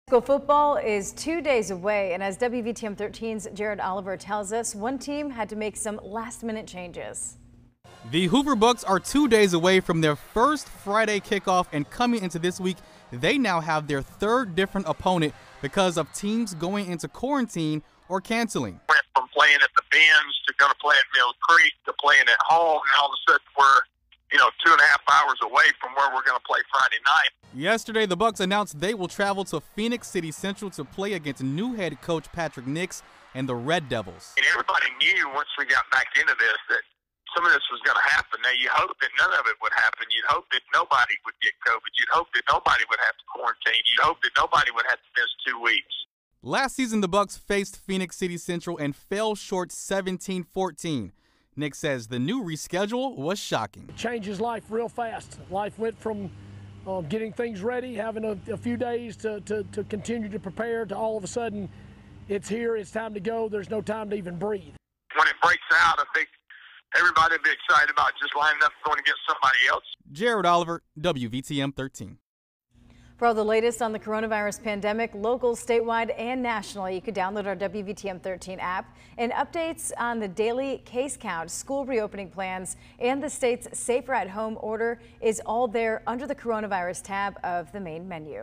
Hoover Bucs turn to Pat Nix, Central-Phenix City to fill hole in football schedule
After some last-minute scrambling, the Hoover Bucs football team managed to find an opponent for the season opener on Friday. Josh Niblett's Bucs will play Central-Phenix City led by new head coach Patrick Nix. Learn more in the video above.